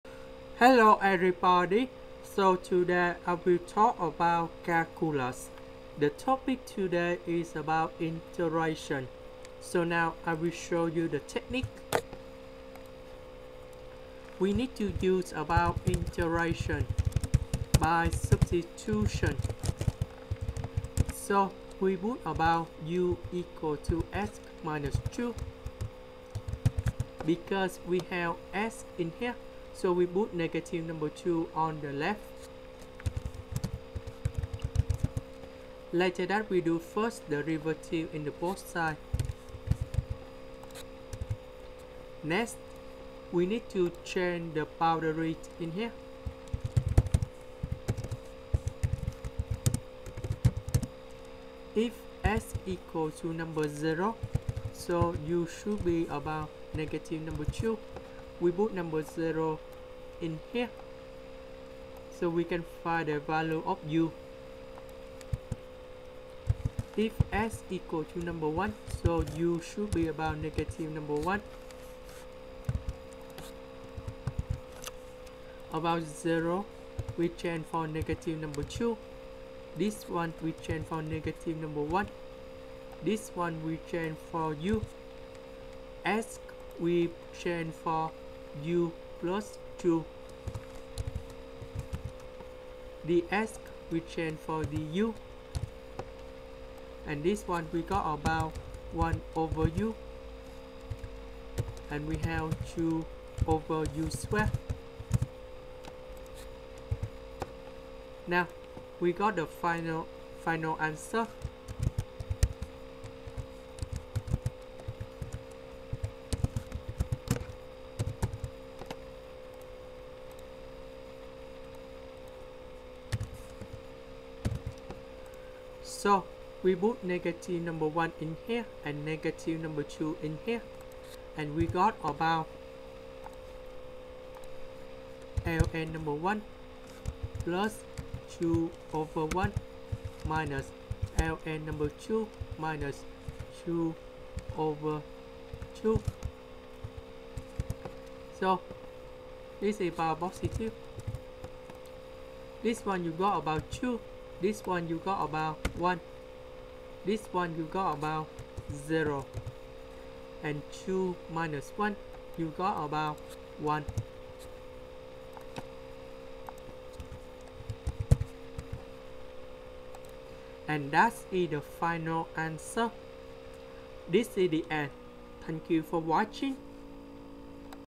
Calculus Help: Integral of x/(x-2)^2 dx - Integration by substitution
Here is the technique to solve this integration and how to find them